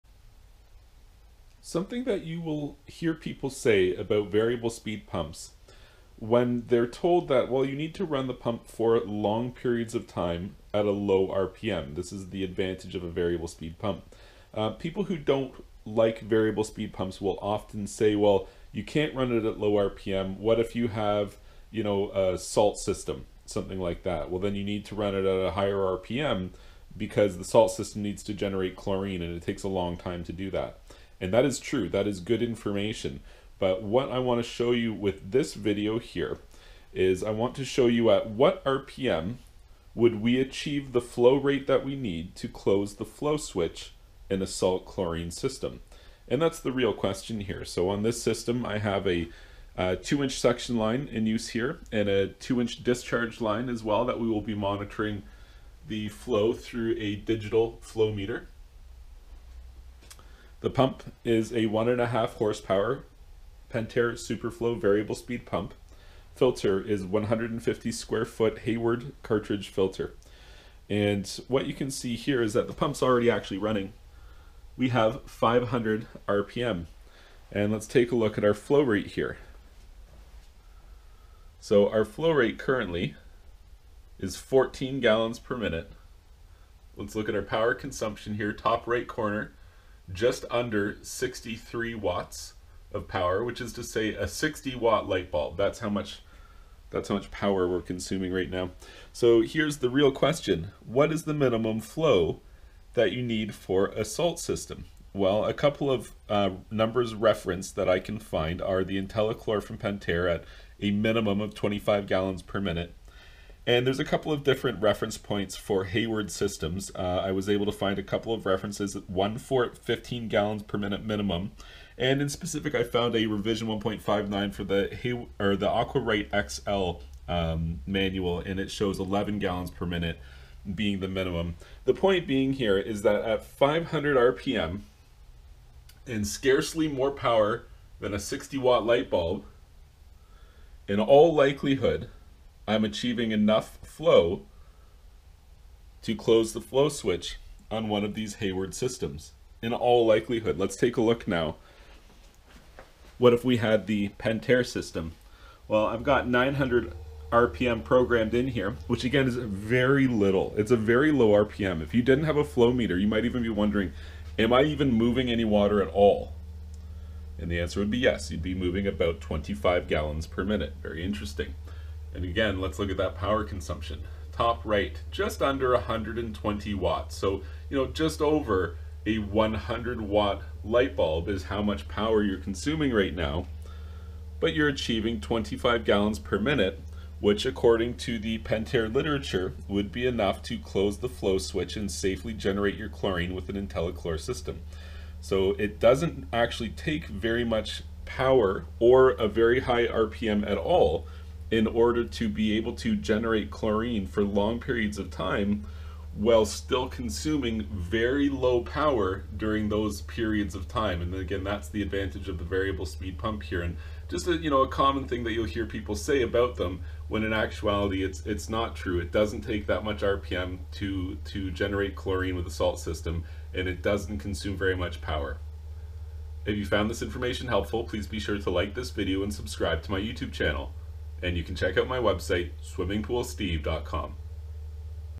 What Pump Speed Do I Need To Run My Salt System?
- See an example of the minimum speed to run you variable speed swimming pool pump to generate chlorine with a salt water chlorinator.  The exact RPM value needed to close the flow switch for a salt water system will change from pool to pool and pump to pump so the manufacturer publishes minimum flow rates (in GPM).  Watch this video to see real world examples of the pump RPM needed for Hayward salt systems or Pentair salt water chlorinators to function.